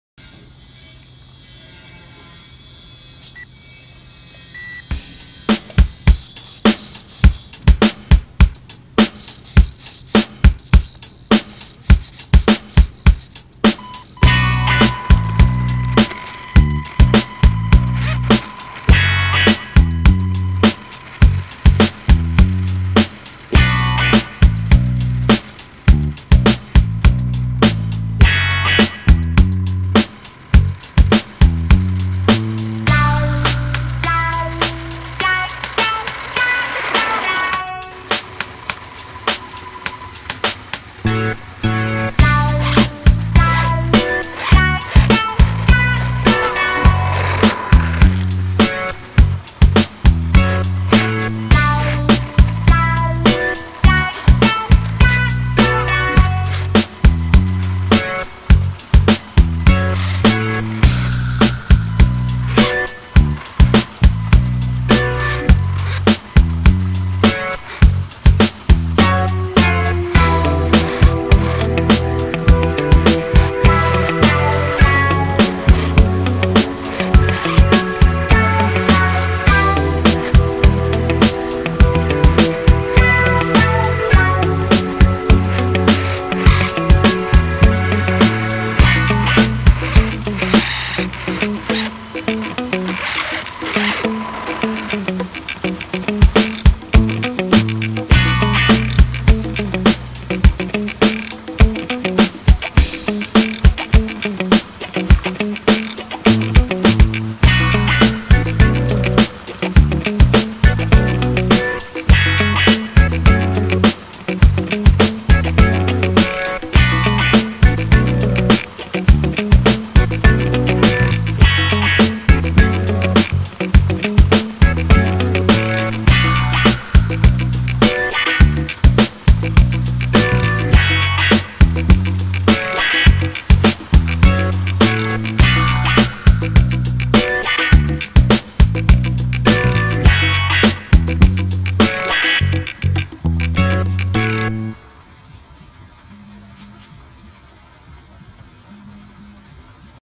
RC Homemade | RC Heavy Truck 6x6x6 OPTiMUS Semi with BEAST II 6X6 Outdoor | Make it yourself
In this video just show you about RC Truck home made working outdoor.